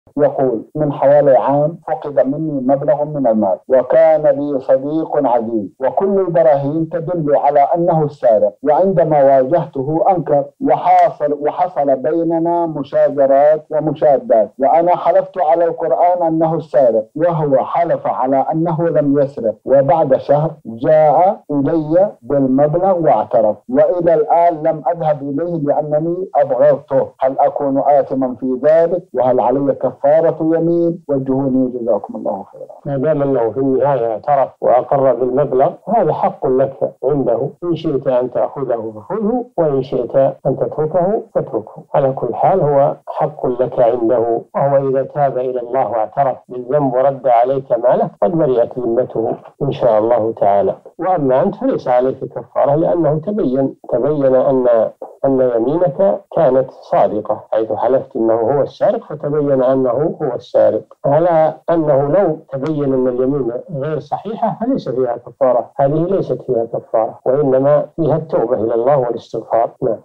شخص سرق مني مبلغا وحلفت أنه السارق ثم اعترف والآن أبغضه بسبب فعله فهل أكون آثما، وهل علي كفارة يمين؟
نص السؤال: من حوالي عام سرق مني مبلغٌ من المال وكان لي صديق عزيز وكل البراهين تدلُ على أنه السارق وعندما واجهته أنكر، وحصل بيننا مشاجرات ومشادات وأنا حلفت على القرآن أنه السارق وهو حلف على أنه لم يسرق وبعد شهر جاء إليَّ بالمبلغ واعترف وإلى الآن لم أذهب إليه لأنني أبغضته، هل أكون آثمًا في ذلك وهل عليَّ كفَّارة يمين؟
السيرة الذاتية :
للاشتراك في قناة الشيخ صالح الفوزان

رئيس الفريق
لمشروع كبار العلماء